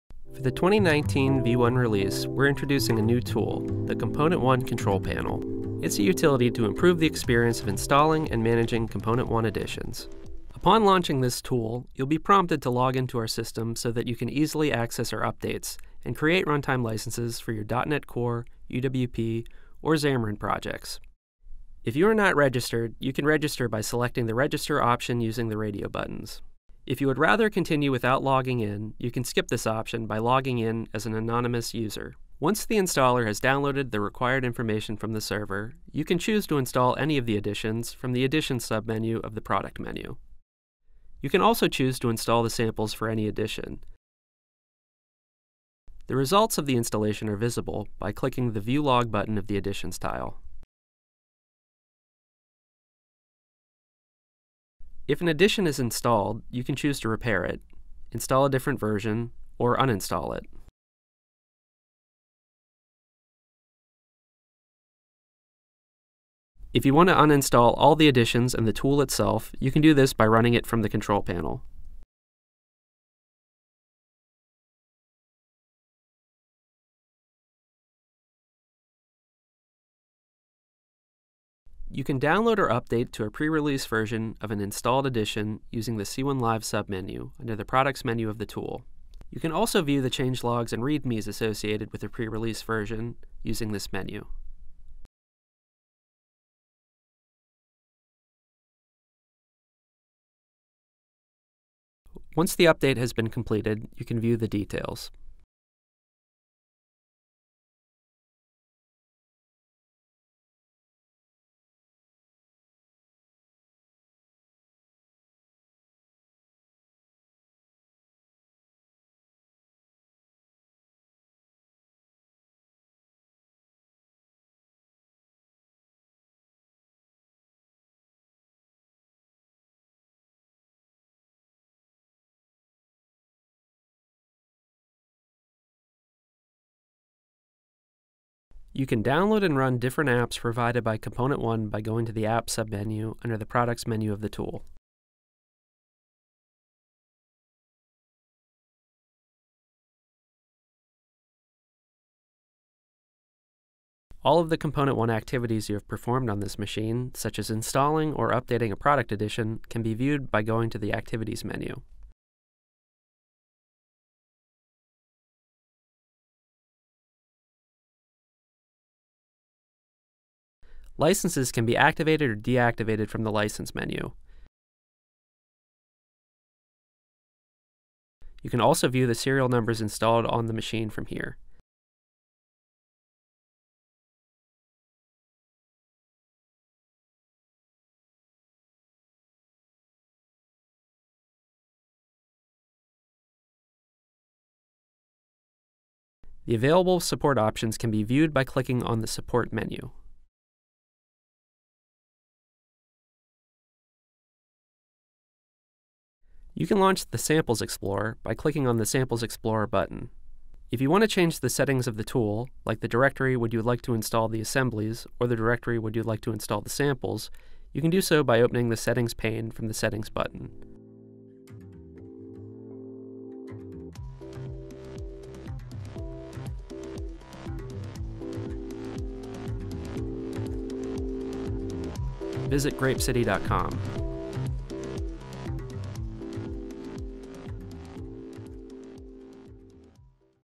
ComponentOne ControlPanel: Installing, Licensing, Updating, and Versioning Your .NET UI Controls
ComponentOne Studio Enterprise has a powerful feature called ComponentOne Control Panel. This tool is designed to improve your interaction with ComponentOne libraries. The new application handles installing, licensing, updating, and versioning the controls on your machine, and it provides fast access to samples and support. The Control Panel streamlines the process of using ComponentOne controls and replaces several disparate apps from the past.

00:00 Explore the ComponentOne ControlPanel
00:32 Use the ComponentOne ControlPanel to Install, License, Update and Version Your .NET UI Controls
03:49 Learn More About Our .NET UI Controls